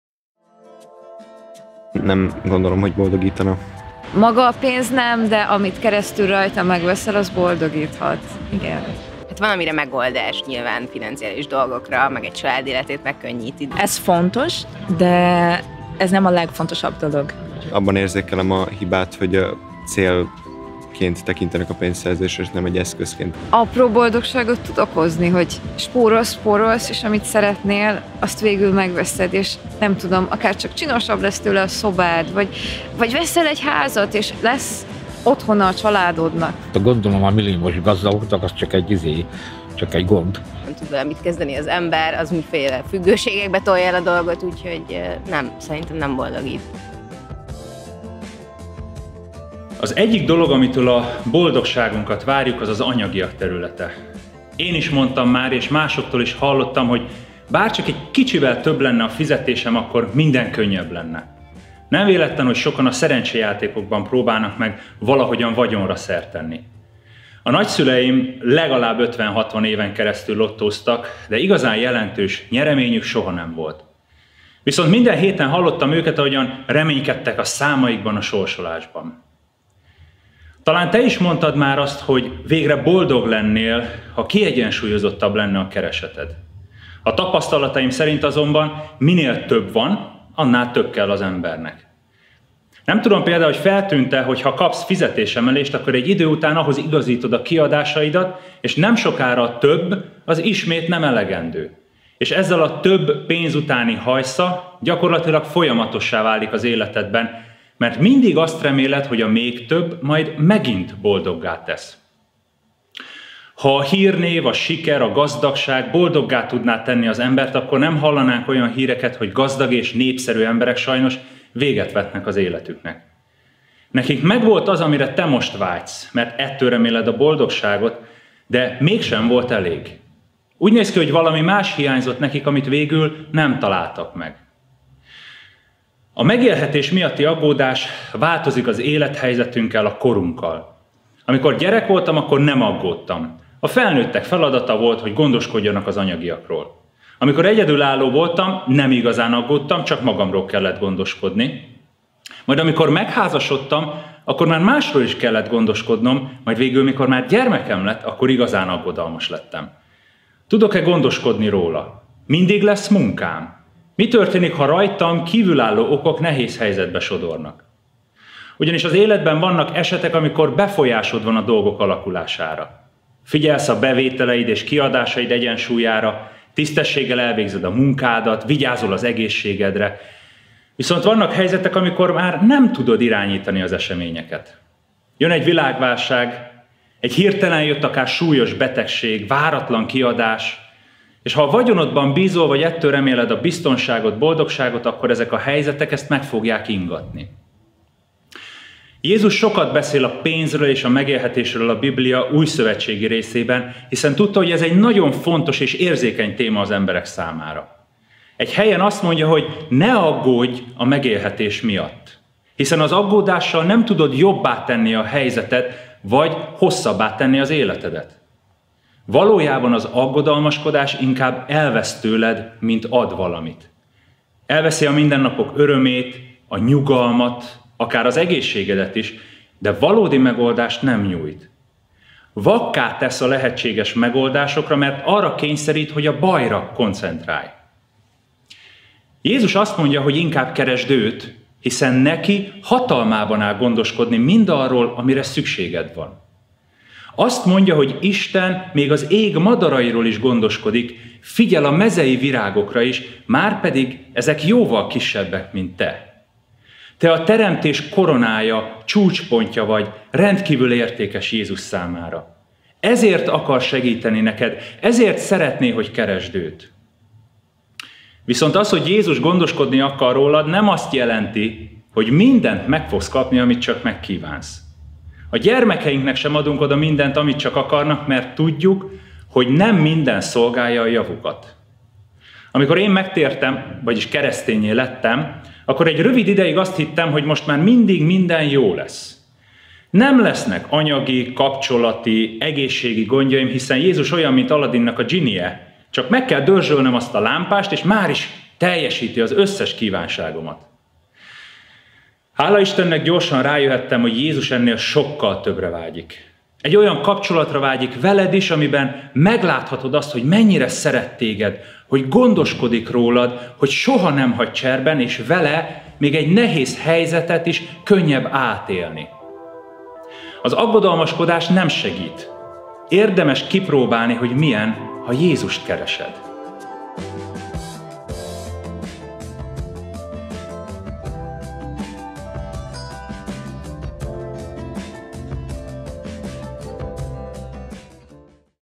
PÉNZ = BOLDOGSÁG?
A pénz boldogít? 🤑 Az elégedett élethez elég ha van pénzünk?💰 Mit mond Jézus? Ha kérdésed van akkor írj nekünk itt vagy keress minket személyesen vasárnaponként! Információ és részletek: golgotabudapest.hu

Does money make you happy? 🤑 Is it enough to have a pleased life if we have money? 💰 What does Jesus say? Information and details: golgotabudapest.hu

Kérdésed van? Keress minket az online platformok egyikén.